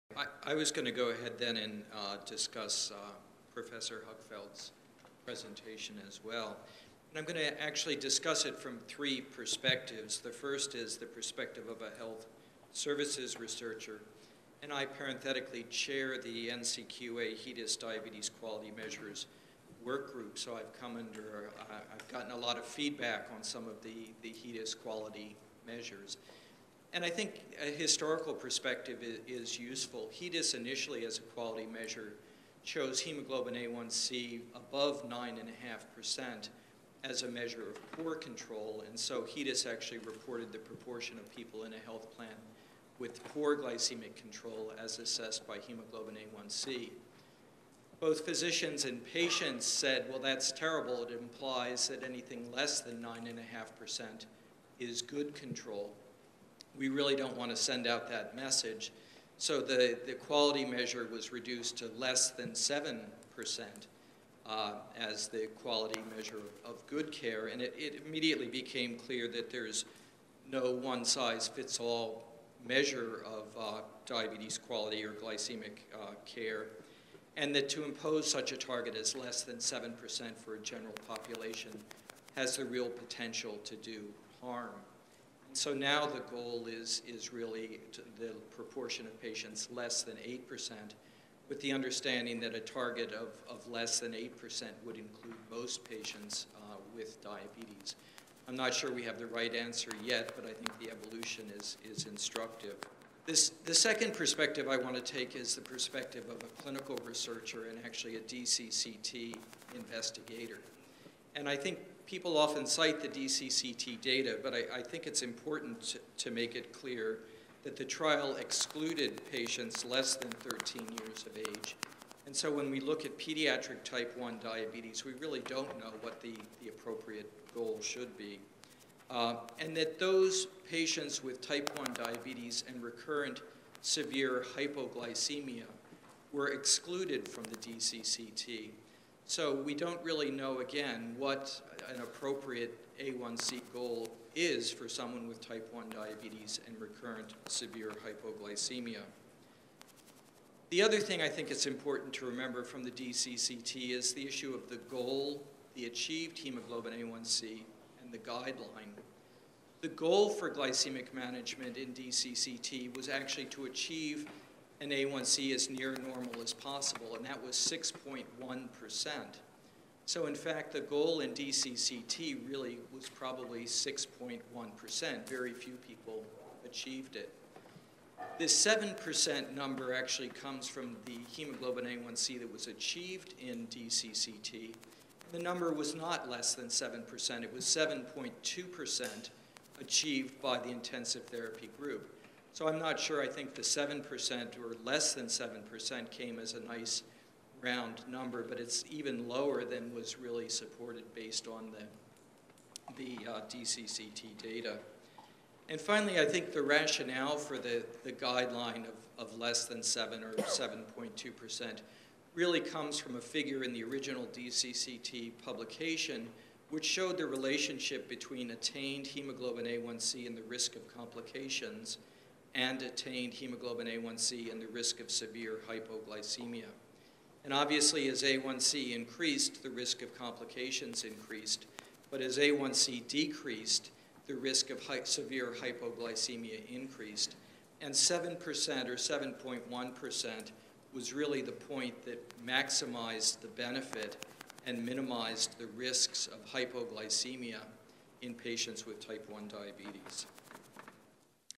William H. Herman, MD, MPH Session 1: Discussant
In this segment, William Herman, MD, MPH, analyzes Dr Huckfeldt's presentation on glycemic targeting from a health service and clinical research perspective. "There is no one-size-fits-all measure of diabetes quality or glycemic care," said Dr Herman. Dr Herman also discussed his viewpoint as a clinical endocrinologist diabetologist. "So patients who are remarkably likely to have targets I think the targets were appropriately modified based on risk of hypoglycemia," said Herman. "I do have general concern that maybe the targets are even too low based on the evidence."